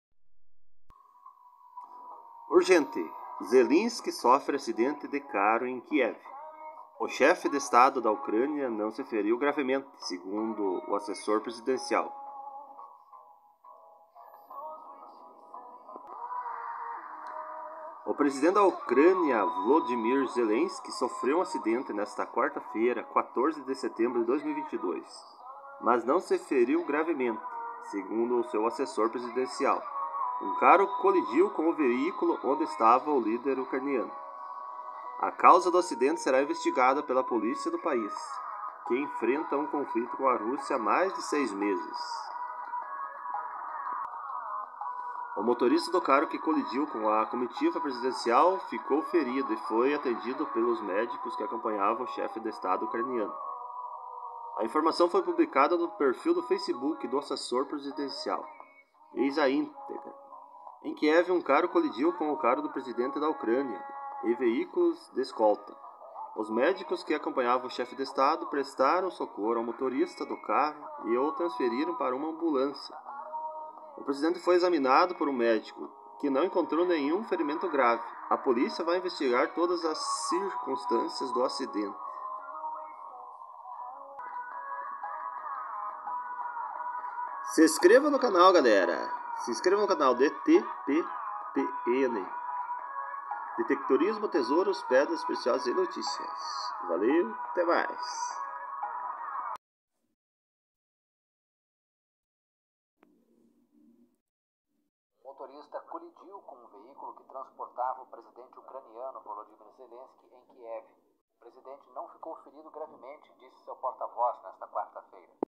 Presidente Ucraniano sofre acidente em Kiev.
Noticia Urgente : O presidente da Ucrânia, Volodymyr Zelensky, sofreu um acidente nesta 4ª feira, mas não se feriu gravemente. Segundo o assessor presidencial Sergii Nykyforov, um carro colidiu com o veículo onde estava o líder ucraniano. A causa do acidente será investigada pela polícia do país, que enfrenta um conflito com a Rússia há mais de 6 meses, veja mais no vídeo.